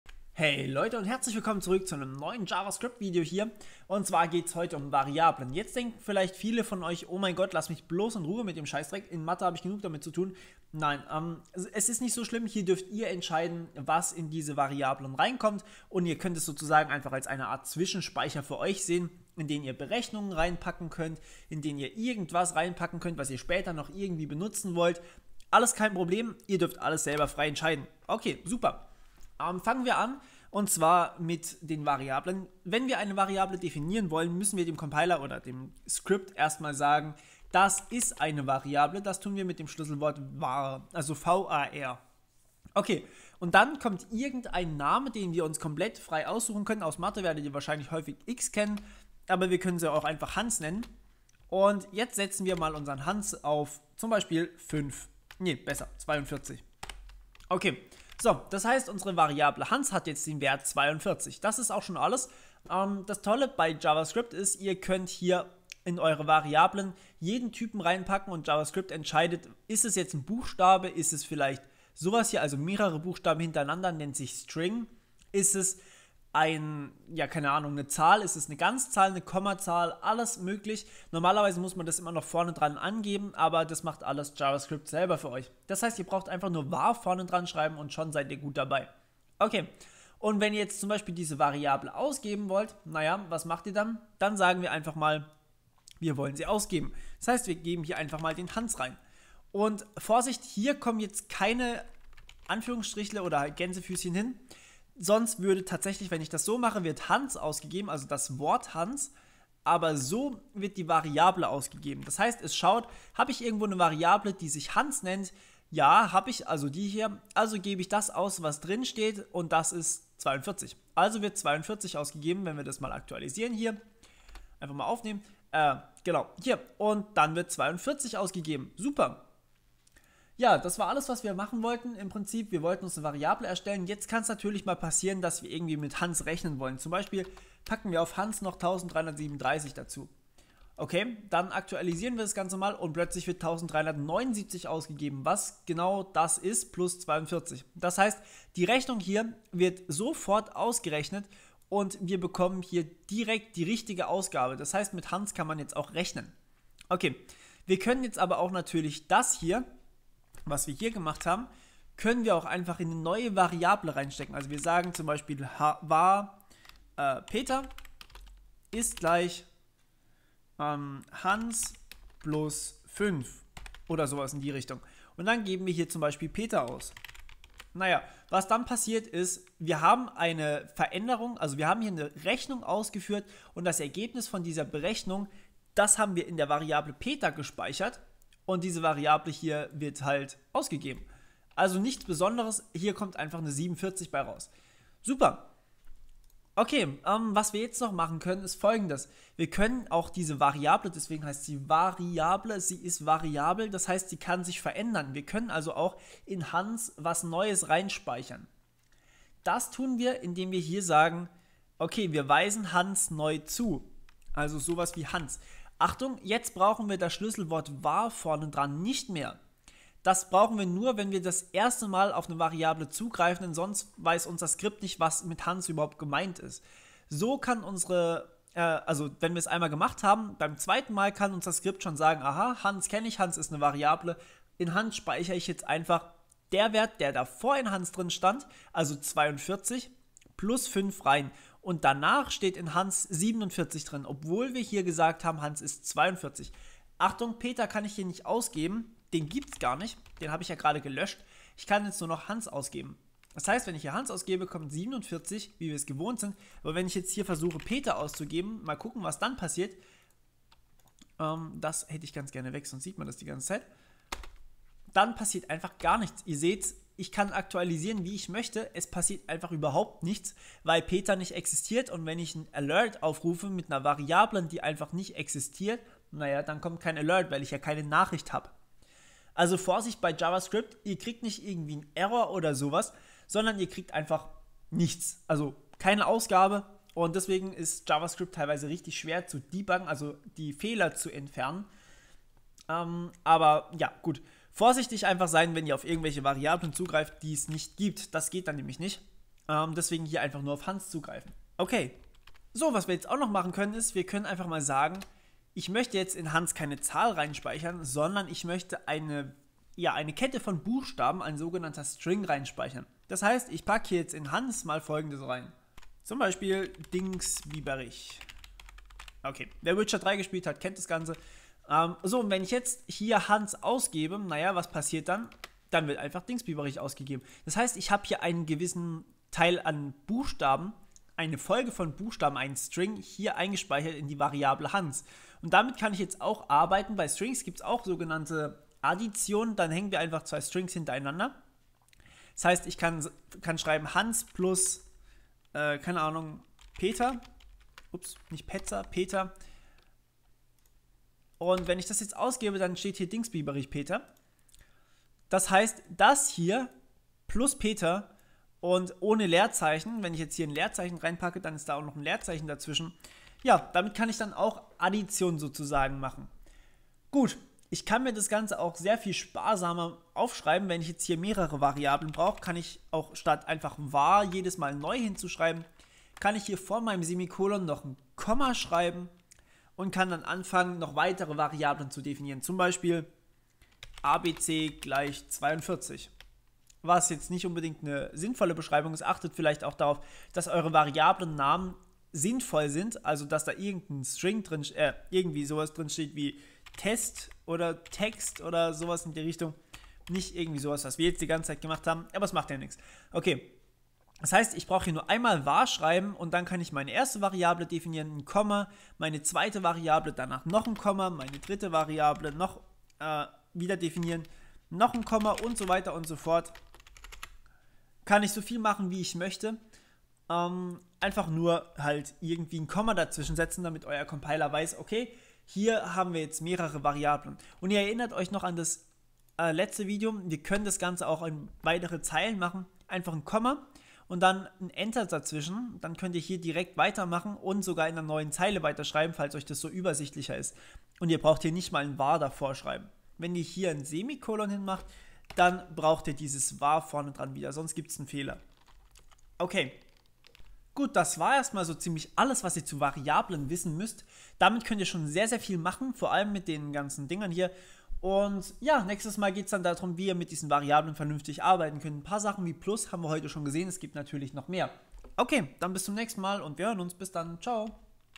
JavaScript Tutorial für Anfänger #3 - Variablen
In diesem Tutorial widmen wir uns Variablen in JavaScript.